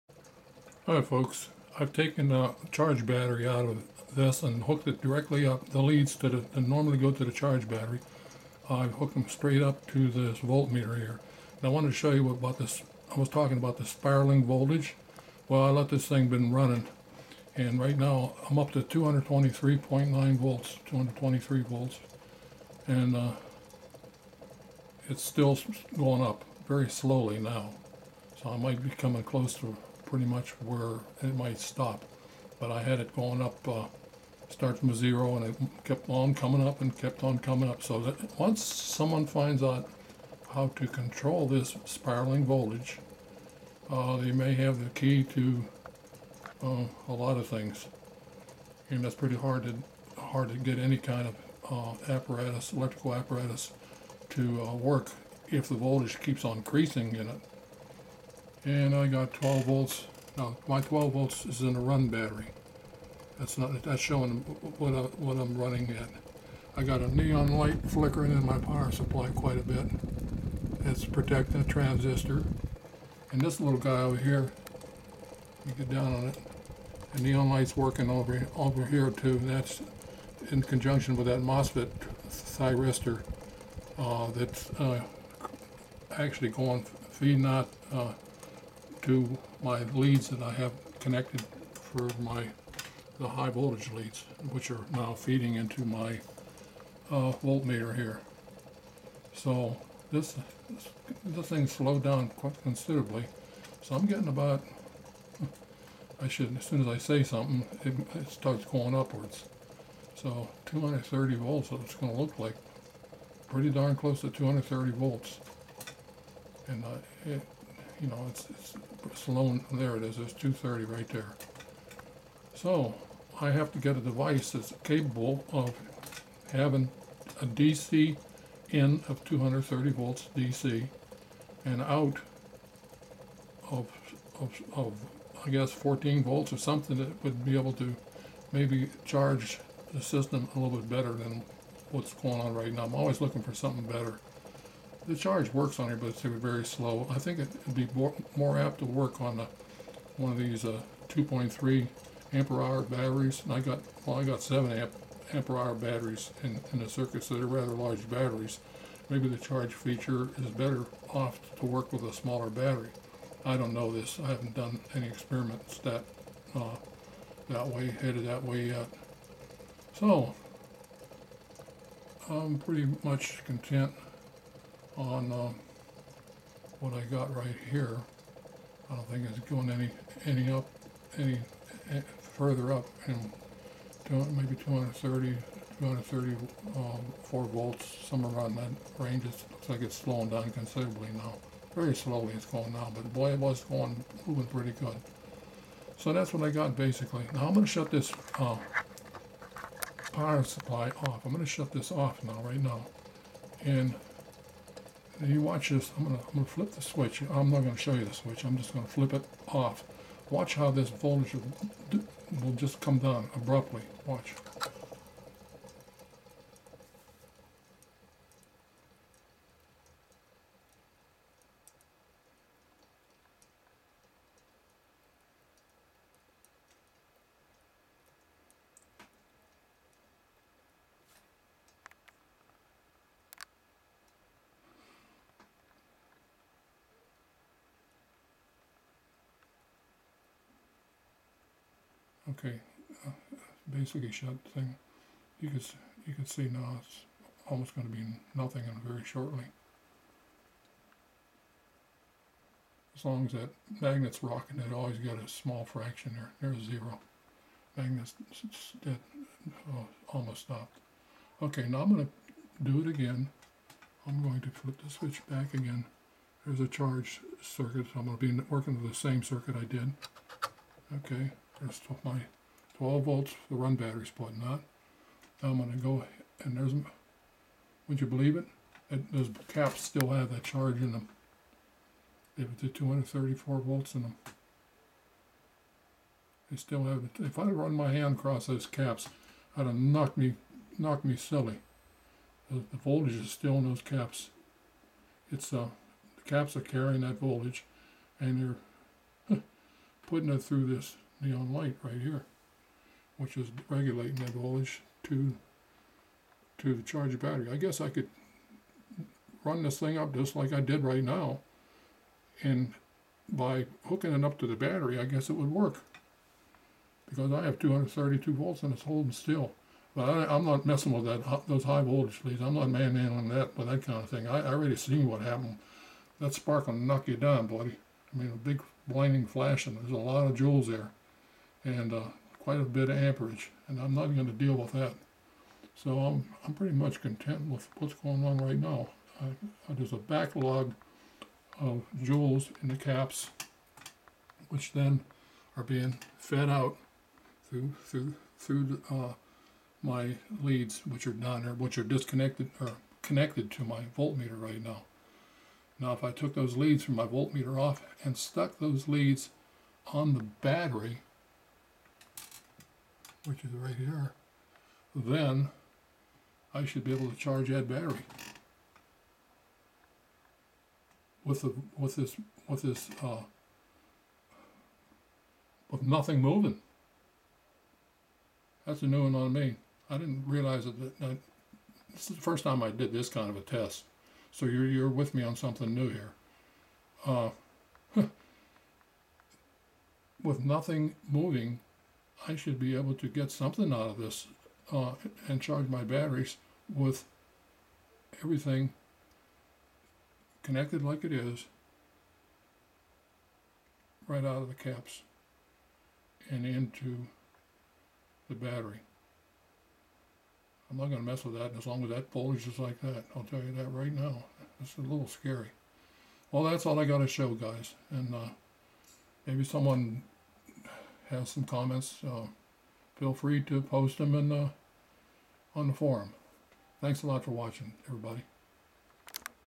VOLTAGE SPIRALING PAST 230 VOLTS
Voltage keeps spiraling on up past 230 volts.
Find a good way to control this  SPIRALING DC voltage and you hold the key to unlimited power.  I just scratched the surface on charging a 12 volt lead acid battery.

Need regulated voltage to charge a battery effectivly.  A cap just stores it like a drinking cup does water  To much water in a cup overflows just like how much power a cap can hold untill it needs dumped.  There has to be a real good way to store and use all that radiant energy without killing yourself in the process.

  In this video when I shut off the power the magnet took its time winding down. Thats were you saw the gradual voltage decrease from 230 to 0 volts.    When  I flipped the switch to run WITOUT GIVING THE MAGNET A FINGER SHOVE You saw the 230+ DC volts the cap still had inside it  Thats the radiant power the caps were storing with NO MAGNET SPIN.

Are you going to be the first one to find  the way to harness all this power??